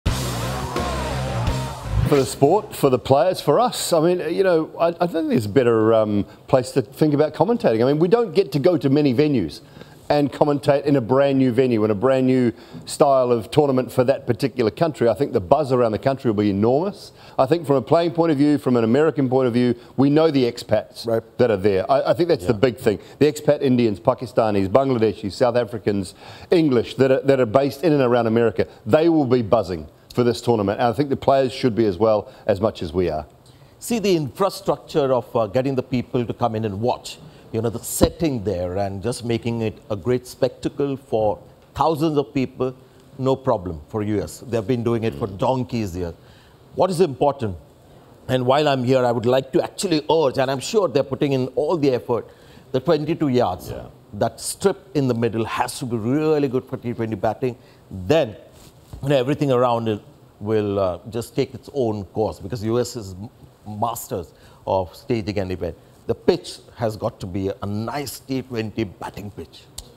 Simon Doull & Sanjay Manjrekar Relish the Cricket Carnival in USA | T20 World Cup
Breaking boundaries on American soil! 🏏🇺🇸

SimonDoull and SanjayManjrekar give the nod to a spectacular T20WorldCup happening in United States! 💪🏻🙌🏻